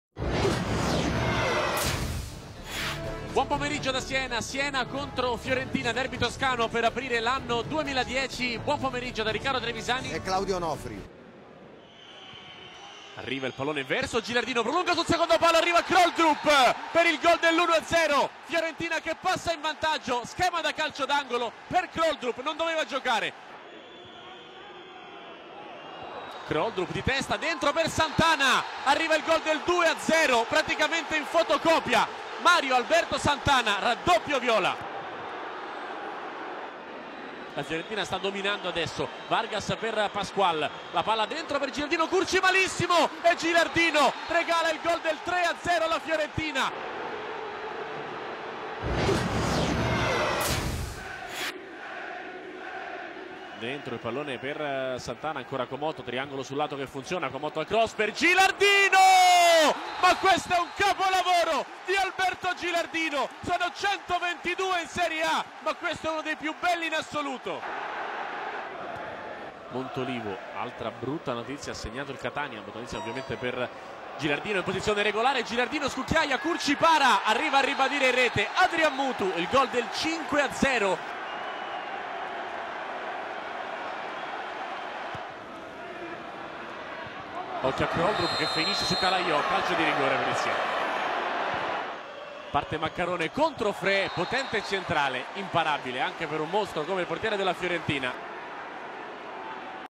Siena-Fiorentina=1-5 (Serie A - 18a Giornata - Goals-Sintesi-Highlights) SKY HD
Pioggia di gol nel derby toscano, ma su 6 gol ben 5 sono dei viola. Partita archiviata in pochi minuti per la Fiorentina che surclassa il Siena per 5-1. Grande prestazione della formazione di Prandelli.